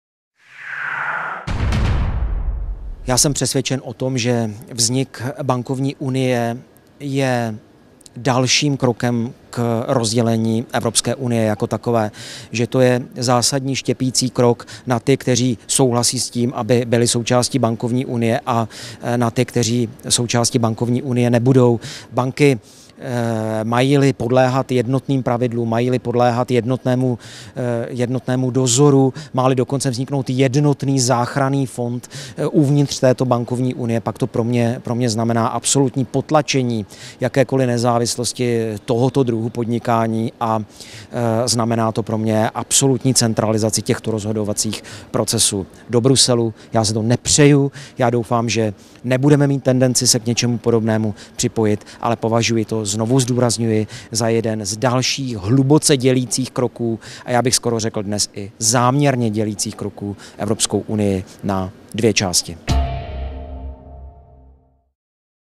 Ivo STREJČEK o bankovní unii
Podle europoslance Ivo Strejčka je vznik bankovní unie jen dalším krokem k rozdělení Evropské unie.